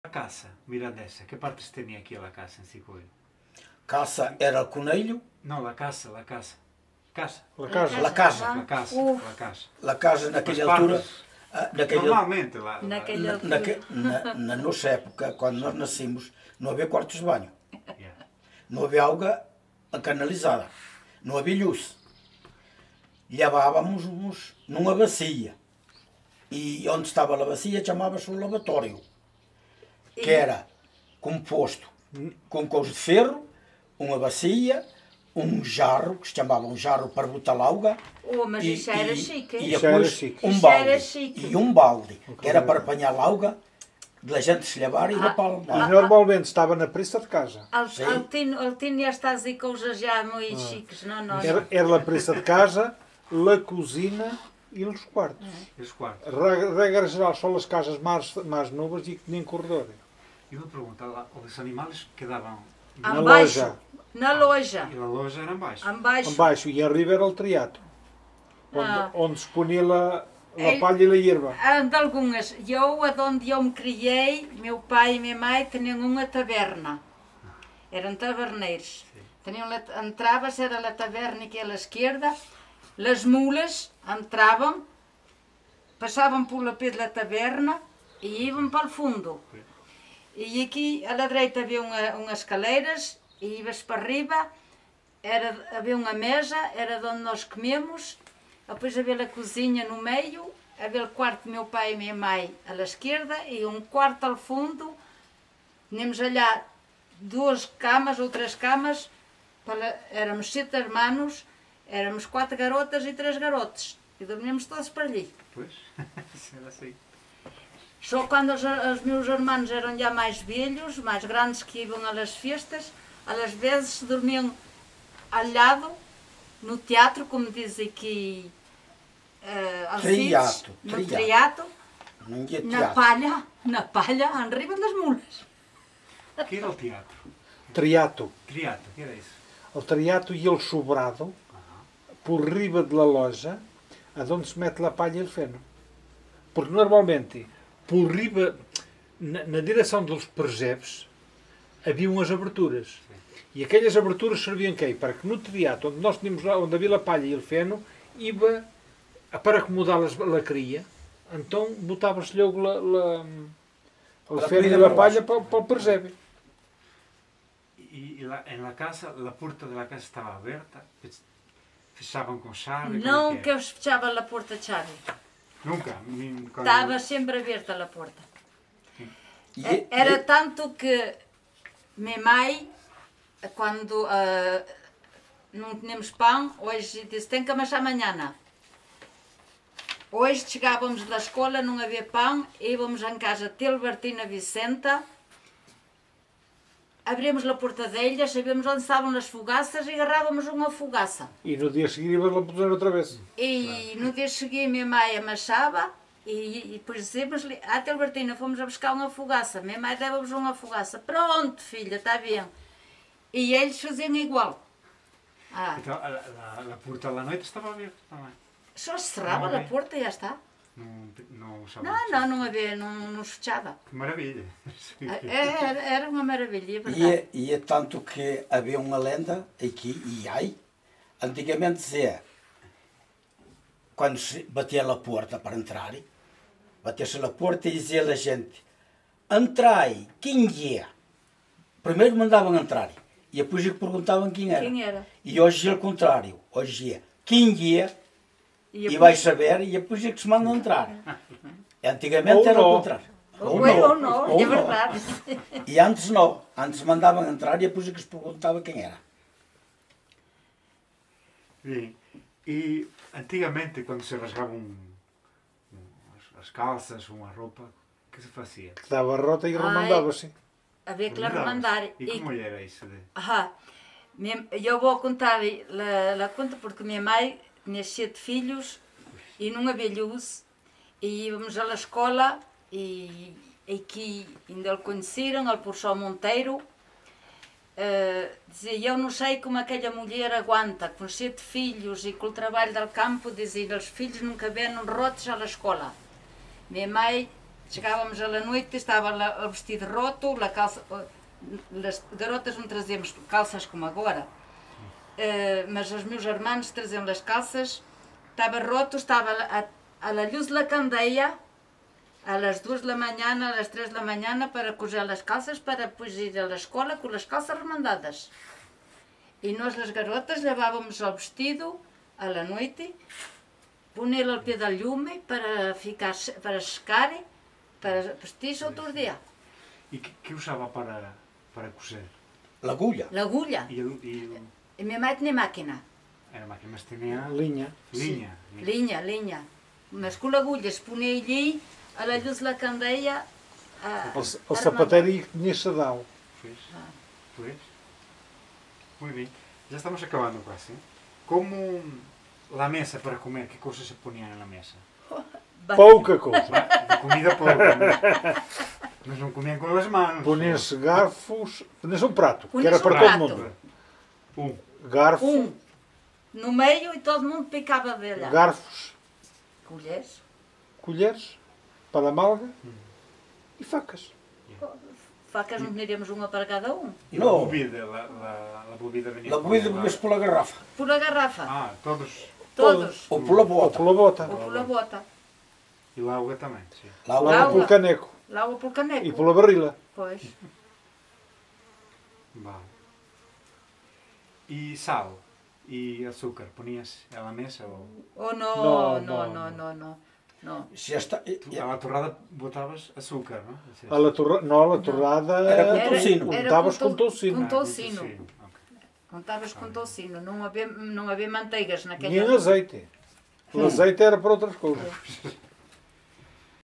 Miranda do Douro 02 (Cicuiro | Cicouro). Sesión 09. La casa
ESP. La casa. Los remiendos. La mesa y los alimentos

PORT. A casa. Os remendos. A mesa e a comida.

ENG. The house. The patches. The table and food.